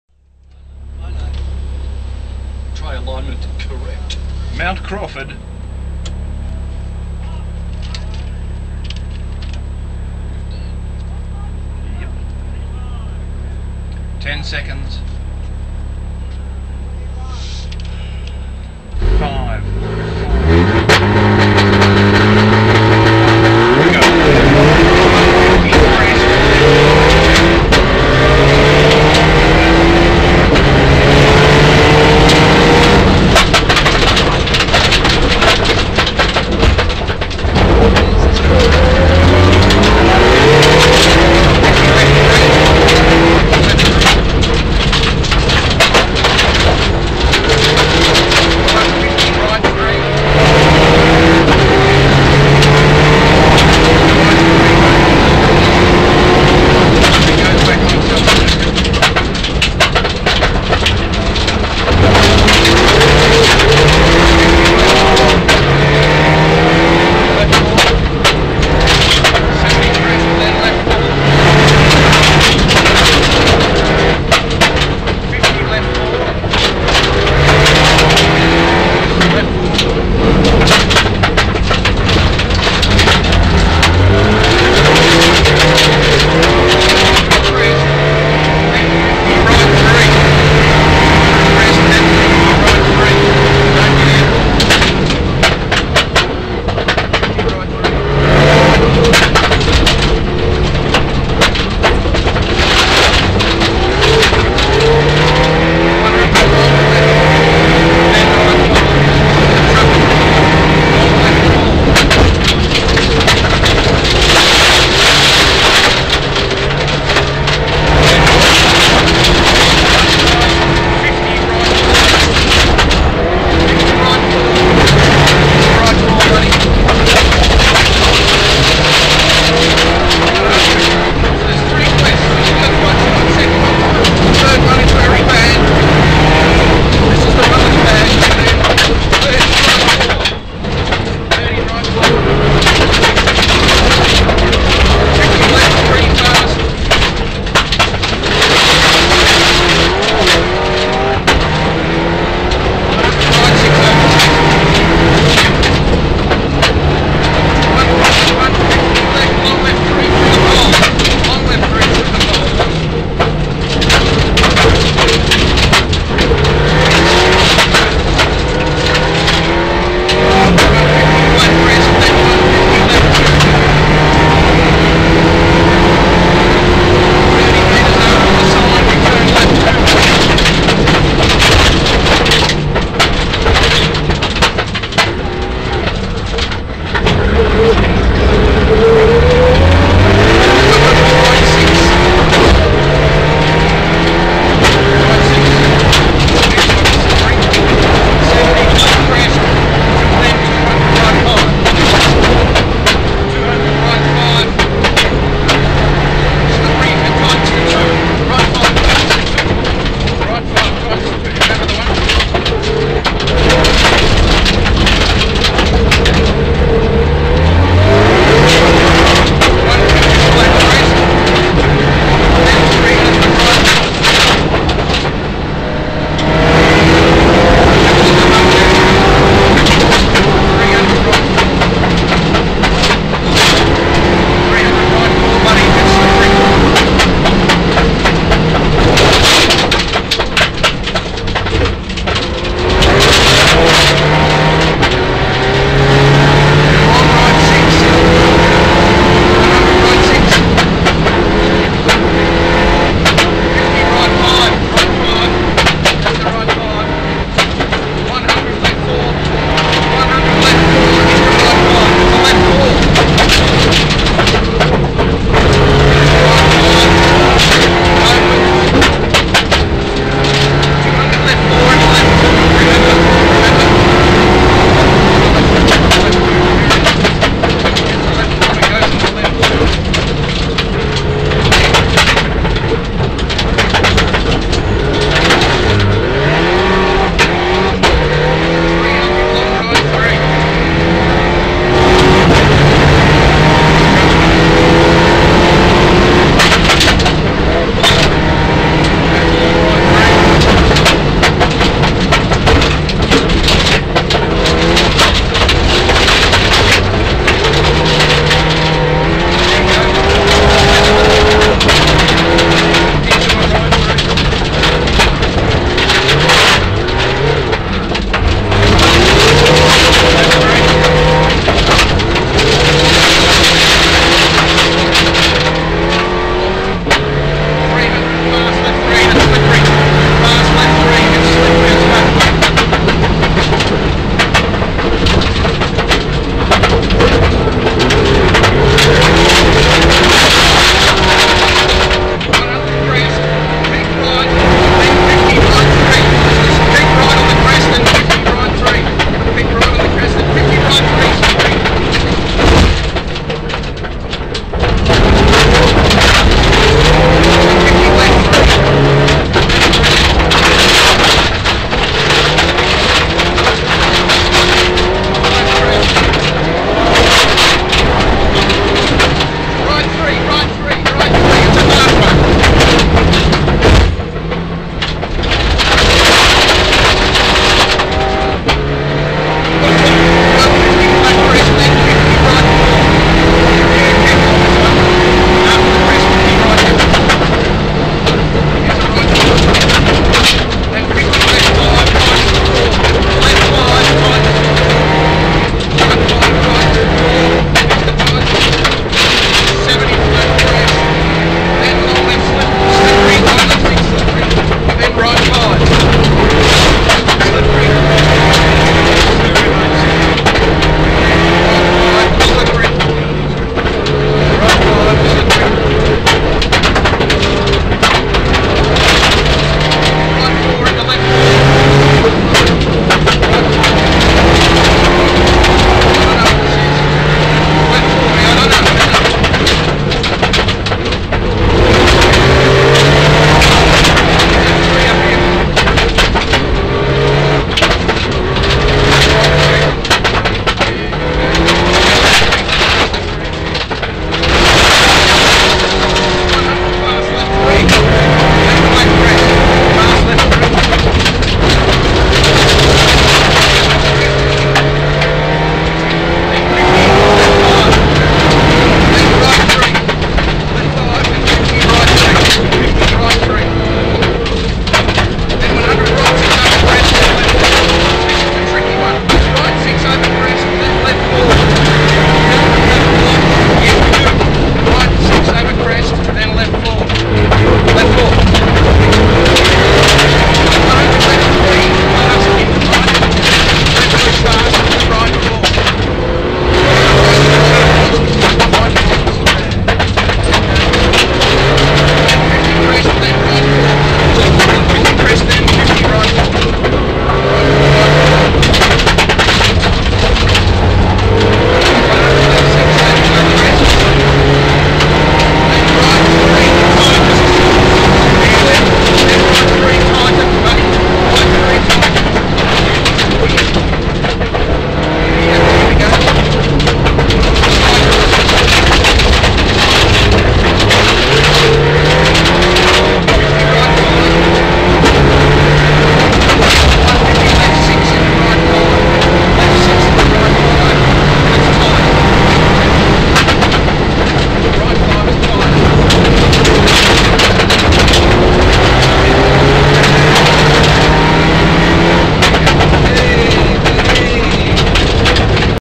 RoSA 2011 SARC SS8 Mt Crawford
Warren Lee and David Lethlean
EVO 9 Car #530
2011 RoSA SARC
SS8 Mt Crawford